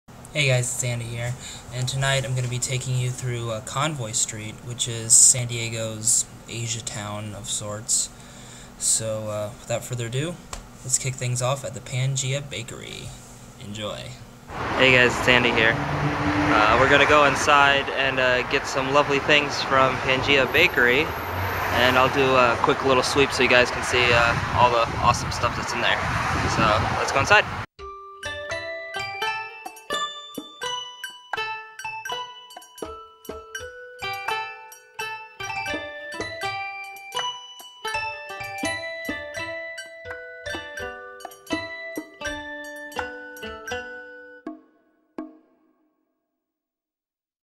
Pangea Bakery - Kearny Mesa 2011 - Part 1-7
Sam and I visit the Pangea Bakery in Kearny Mesa!

These series of videos were all recorded in the Kearny Mesa area of San Diego on September 03, 2011.

TheAndySan

**Tech Notes**

*Program Used*

*Camera Used*

Sanyo Xacti SH-1

*Music Credits*

BGM Artist: Pompoko